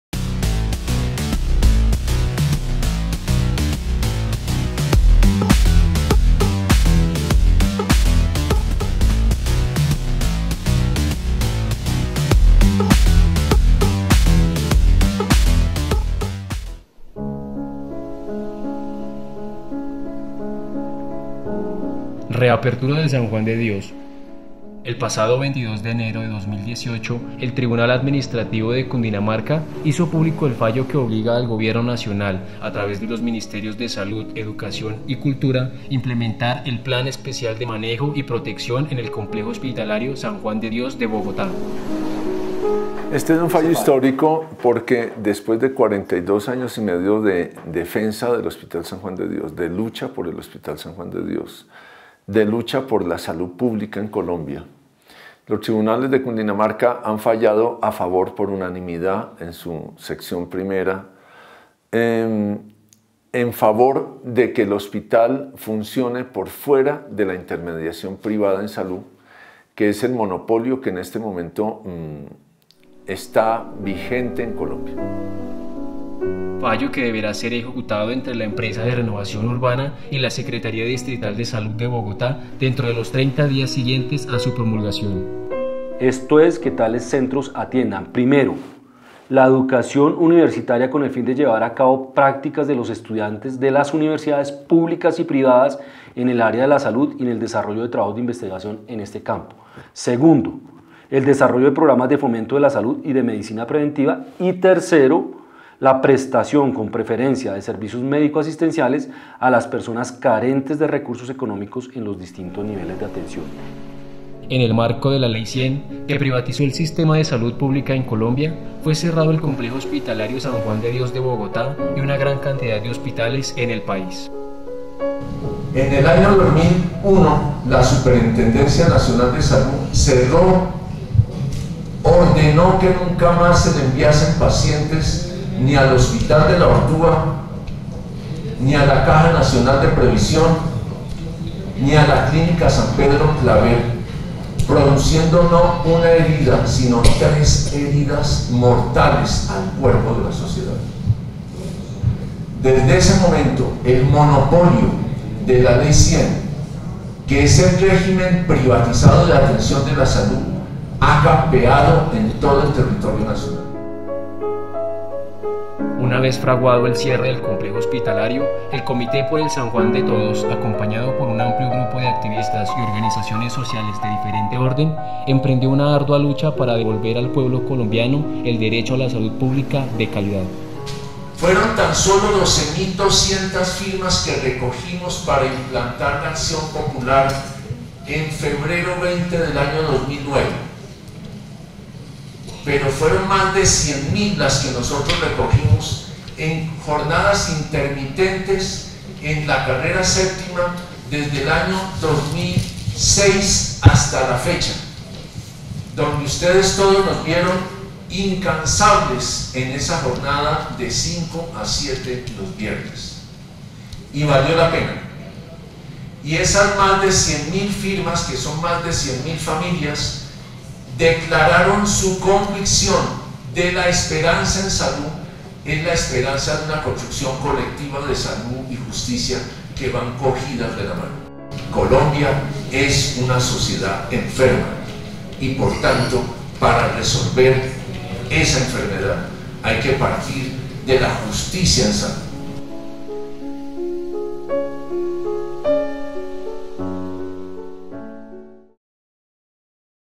El San Juan sí es de todos
Tras 17 años de estar cerrado el Hospital San Juan de Dios de Bogotá, volverá a estar al servicio de la sociedad colombiana. Así lo ordena un fallo del Tribunal Administrativo de Cundinamarca.